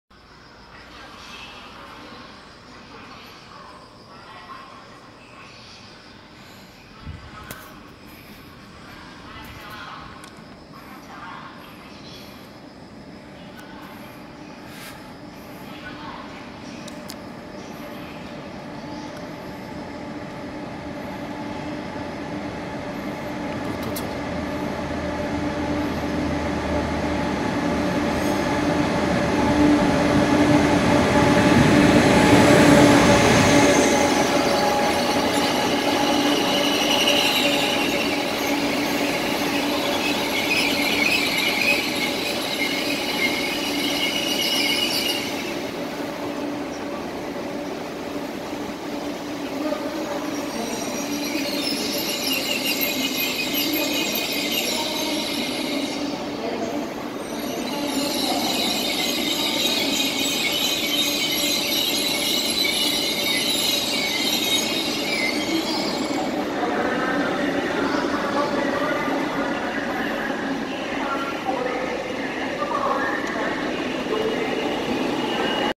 180905 오송역 SRT 수서행 도착
Station : Osong

Train : 1C3007 #622 SRT Suseo